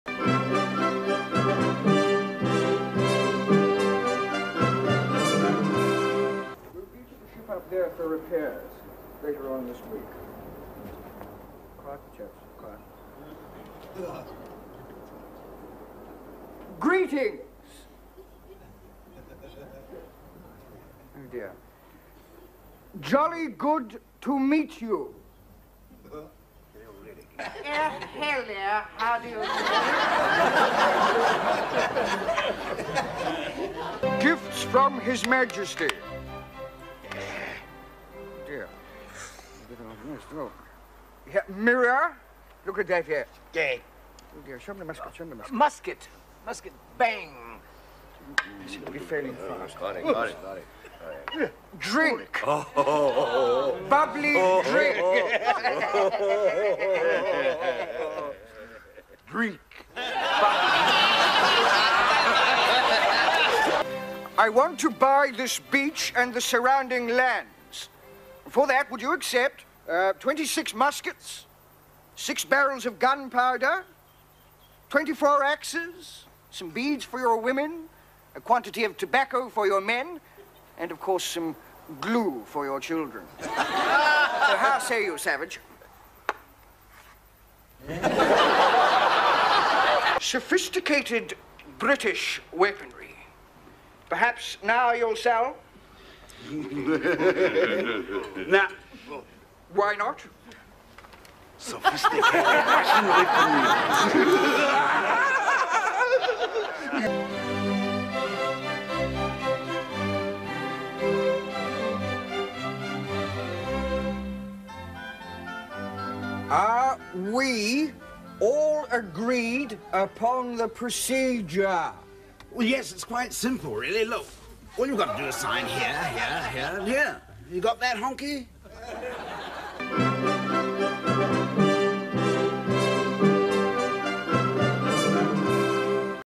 Billy T James Signs The Treaty Of Waitangi
A History of New Zealand.
The Treaty of Waitangi is Signed.

Billy T James, a classic brilliant NZ comedian artist, sadly missed. The world is probably far too woke for his humour today, but he’s still incredibly funny.